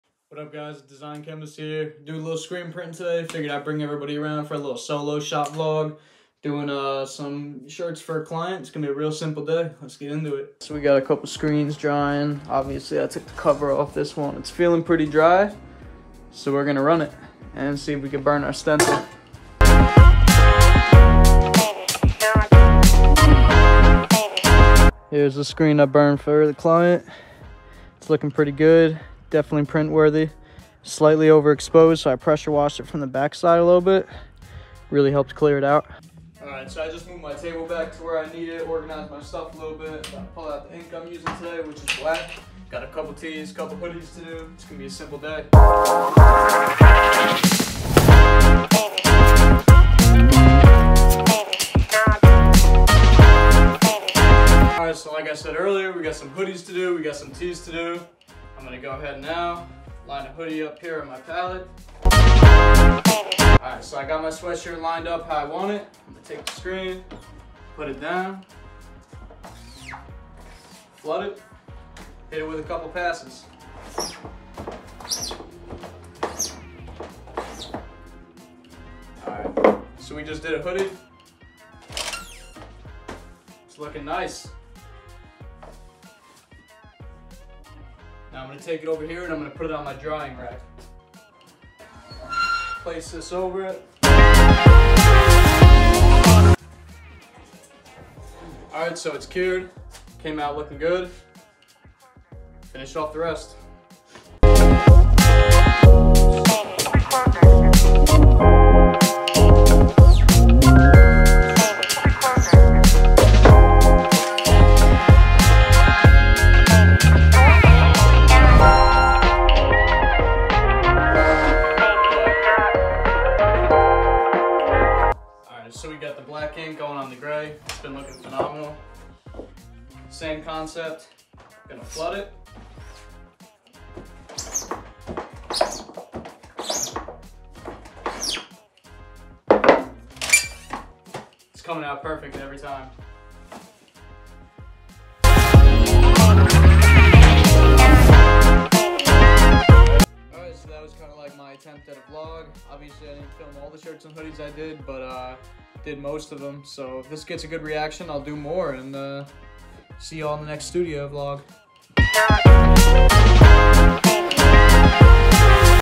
SCREENPRINTING VLOG 2024
Lil studio vlog with the Design Chemist! If this gets a good reaction I’ll make more and dial it up!